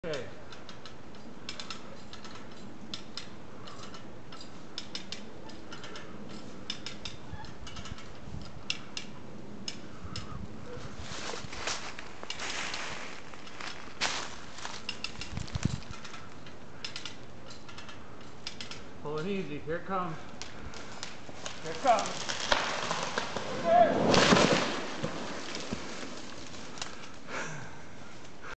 Cherry down
Marty filmed as I brought the huge Cherry tree down.